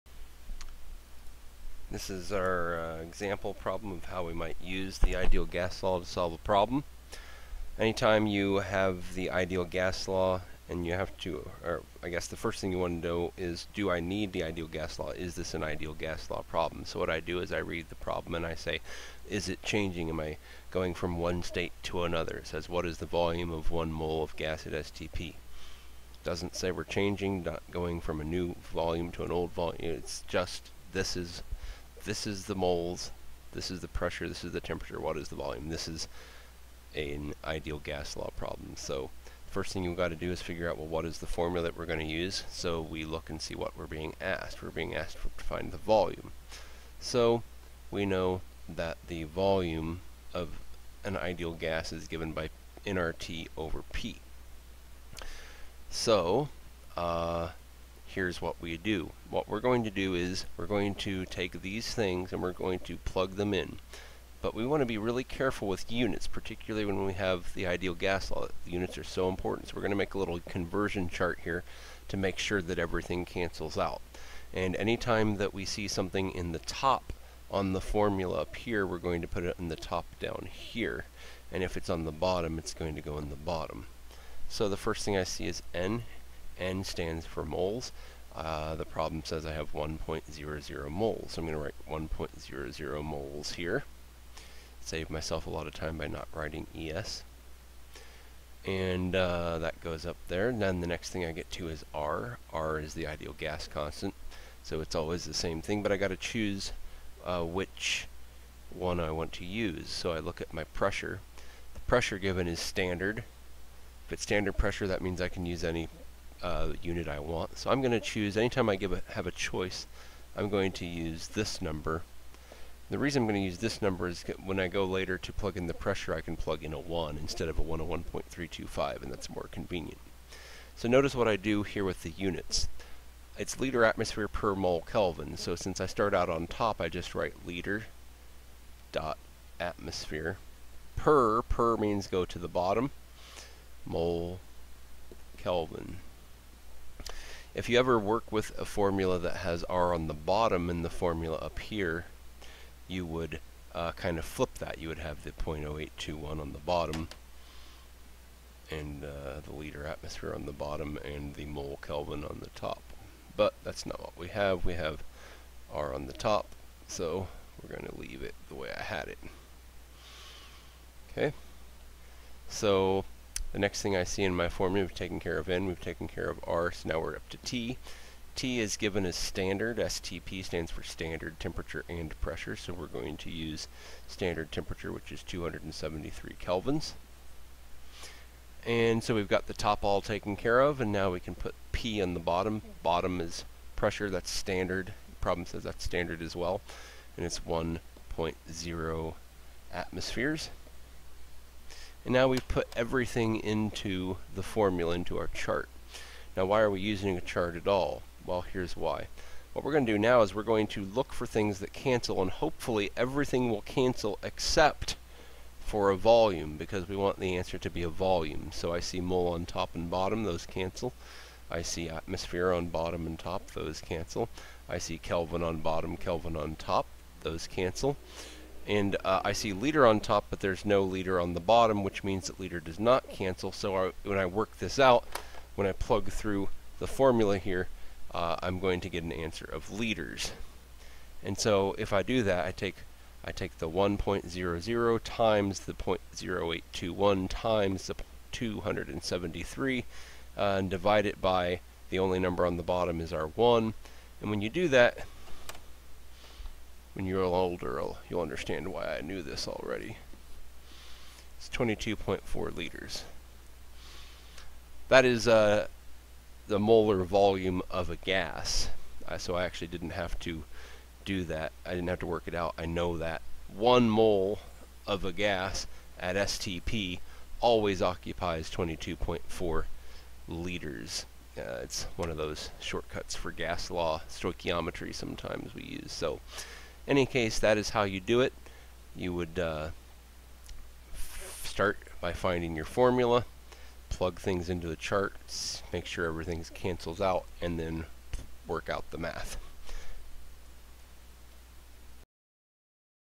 Ideal Example: Find volume of one mole of a gas
How to use the ideal gas law to solve a word problem.